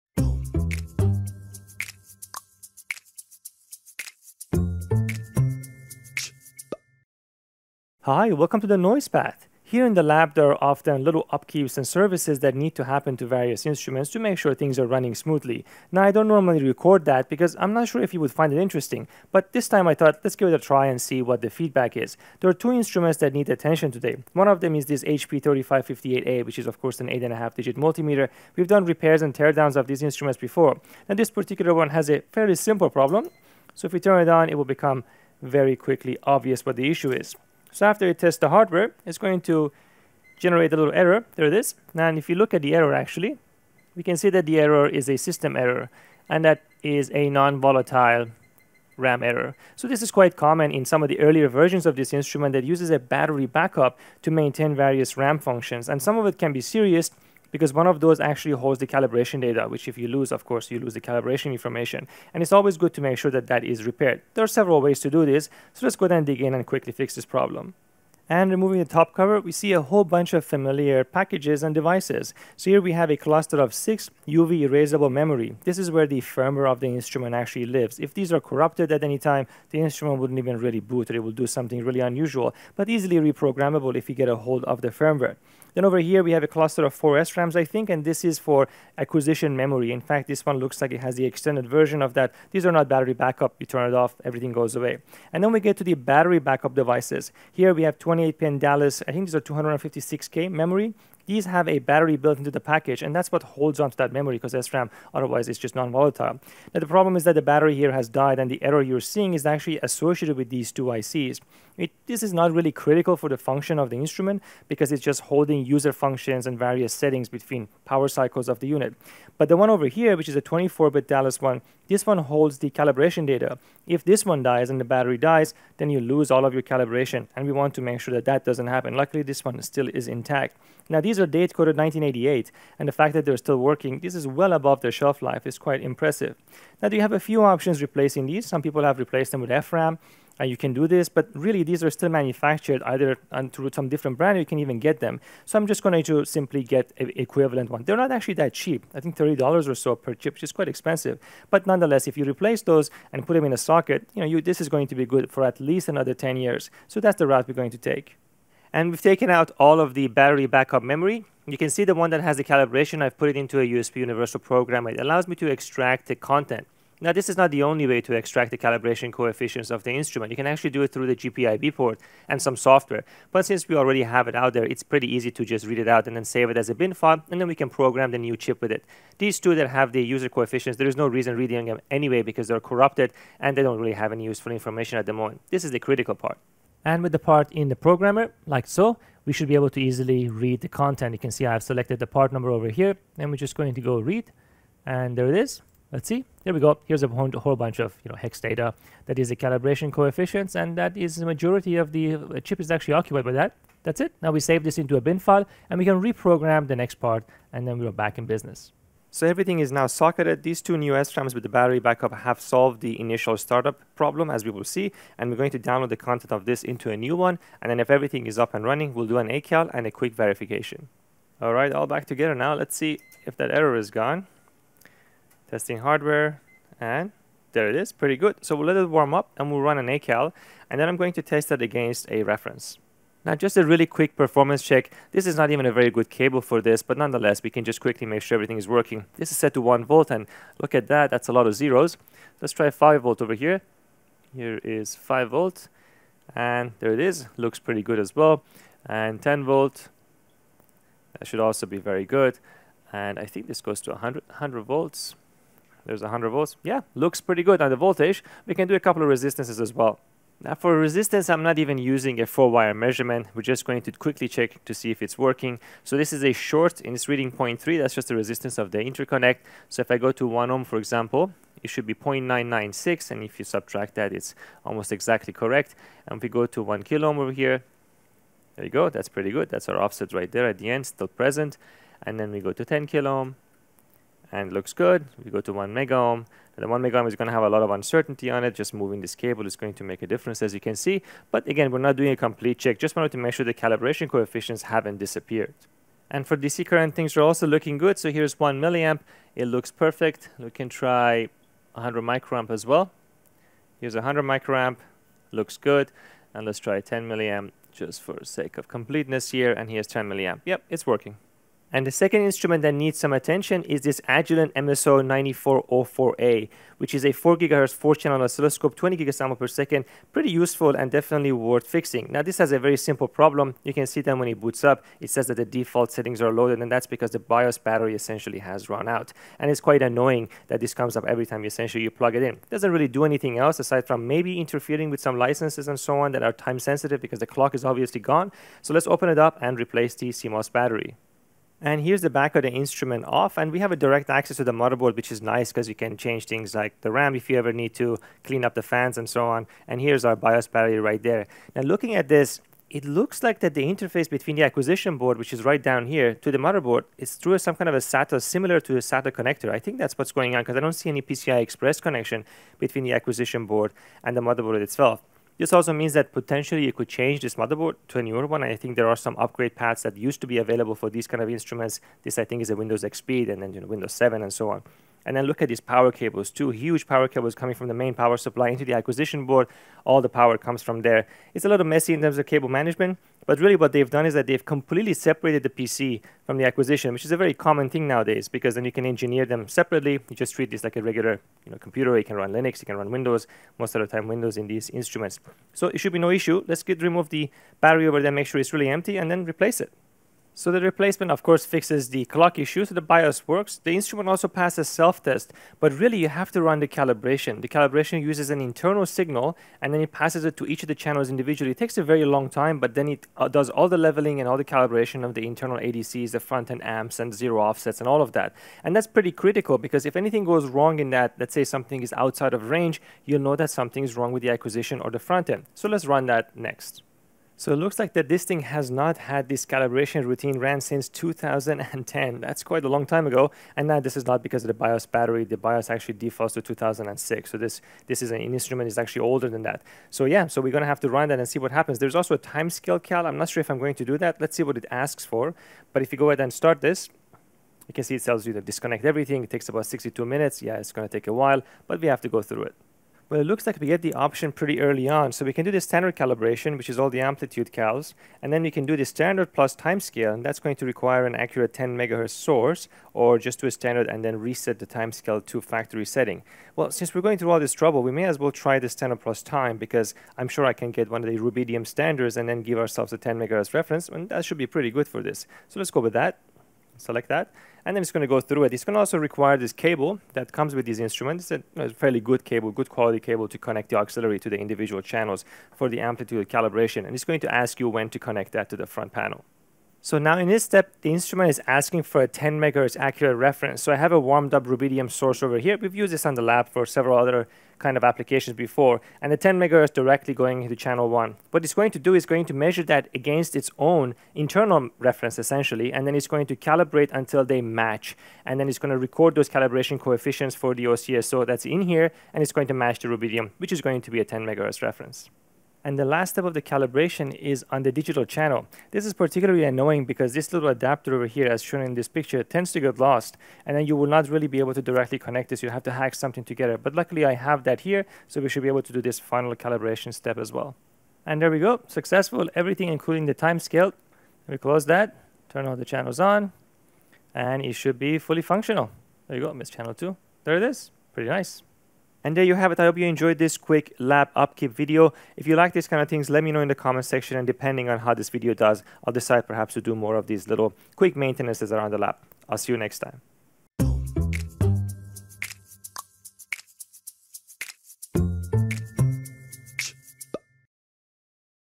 TNP #55 - Lab Upkeep Time! Agilent 3458A 8.5 Digit Multimeter & MSO9404A 4GHz, 20GS/s Scope Repairs
In this quick episode Shahriar performs some regular lab maintenance. Two instrument need attention. The Agilent 3458A is reporting a Nonvolatile Memory Error & and the Agilent MSO9404A is reporting a dead CMOS battery.

Both problems are easily addressed by changing the battery backup. In the case of the 3458A, the calibration content has to be re-programed into the new NVRAM ICs.